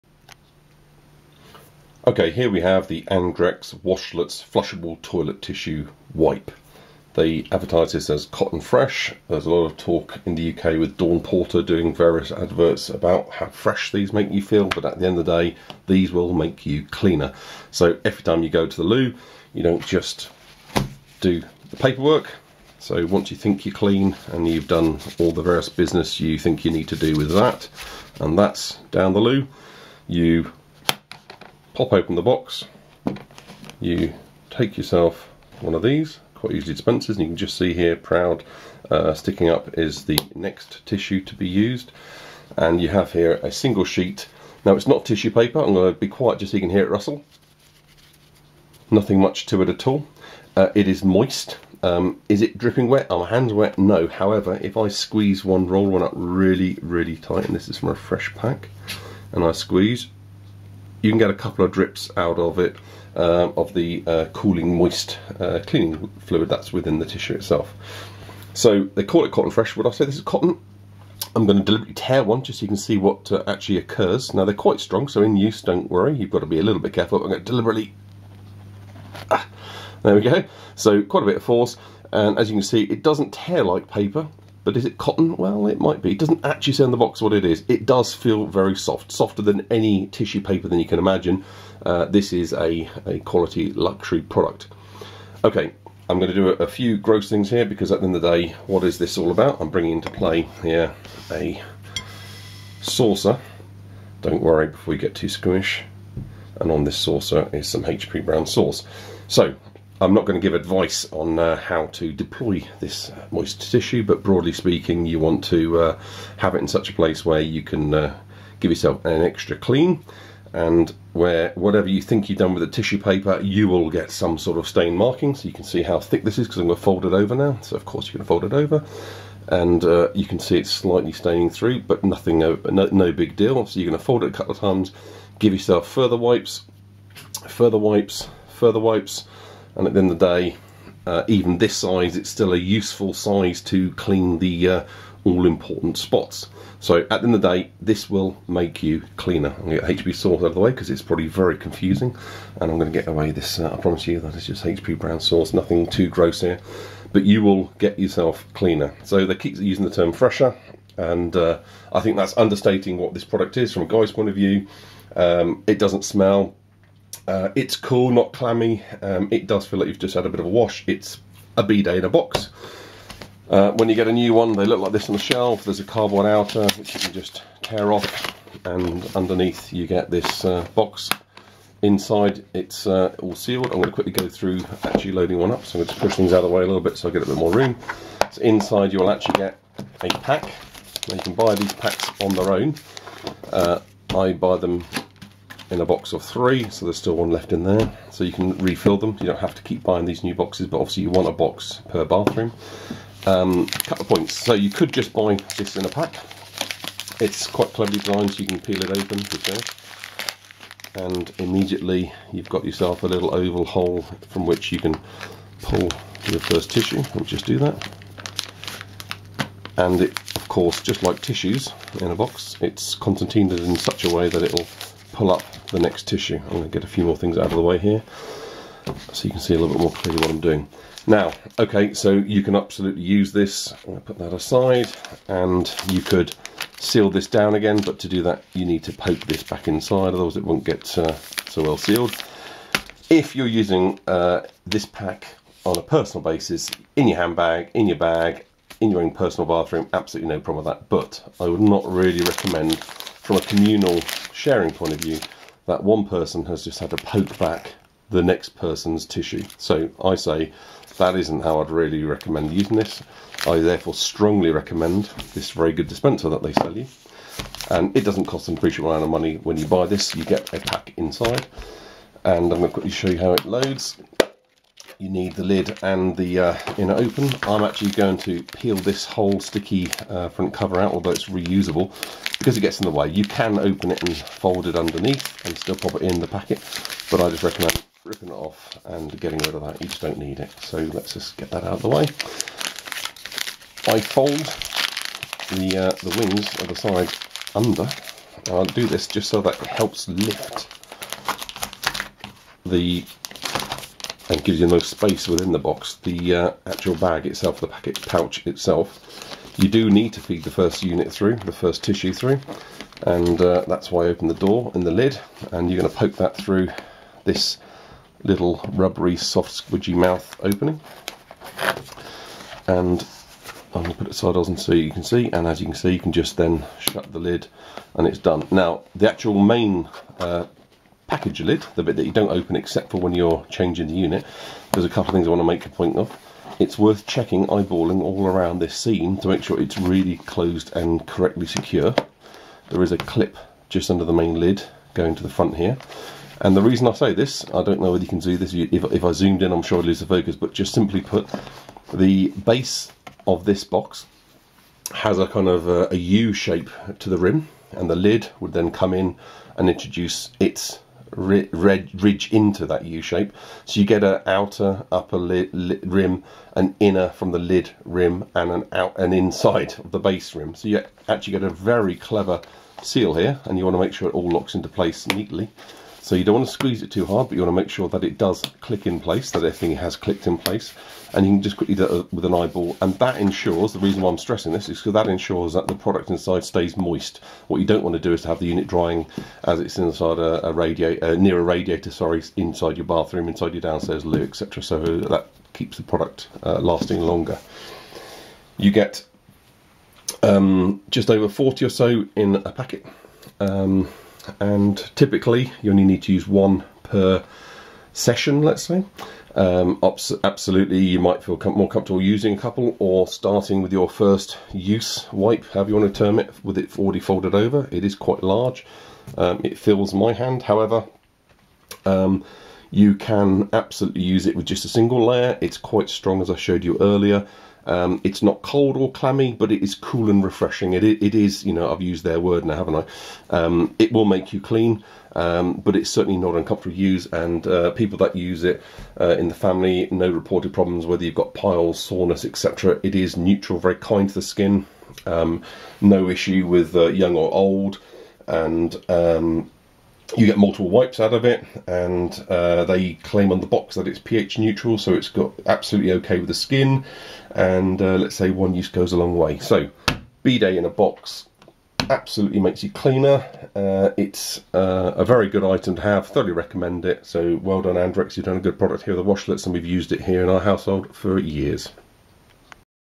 Andrex Washlets - Hands on detailed review. Bidet in a box.
These are moist tissues that you use each time you go to the toilet.
After using normal toilet paper you use one of these Washlets and further clean yourself.